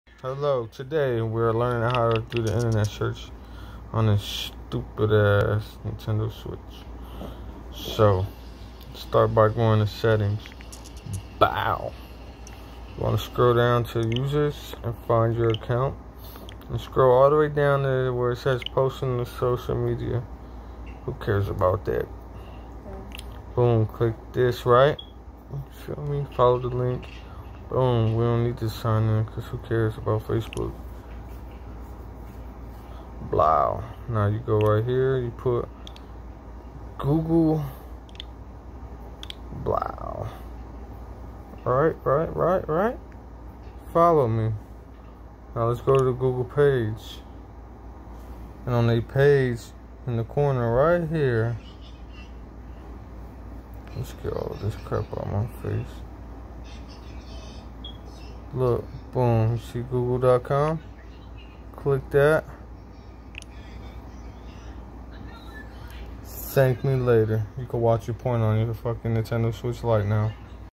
How to search the internet on a Nintendo switch
Igot a Nintendo switch lite and im teaching you how to search the internet